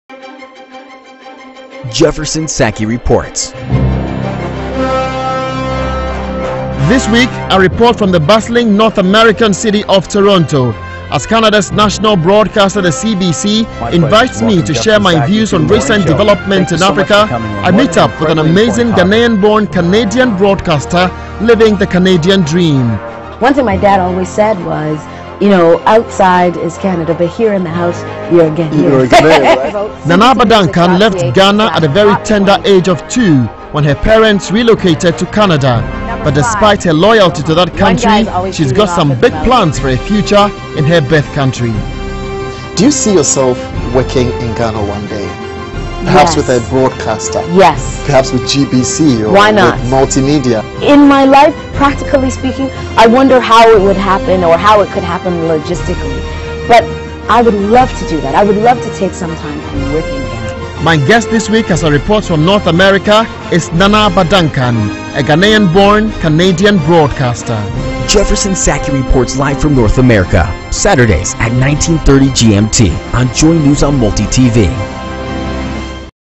JSR NANA ABA DUNCAN PROMO AVI
Jefferson Sackey hosts Nana Aba Duncan a Ghanaian born Canadian broadcaster in the studios of CBC in downtown Toronto. Nana Aba Duncan is the host of Radio 2 Top 20 on CBC. They talk about her job, her ambition and her experience in the job.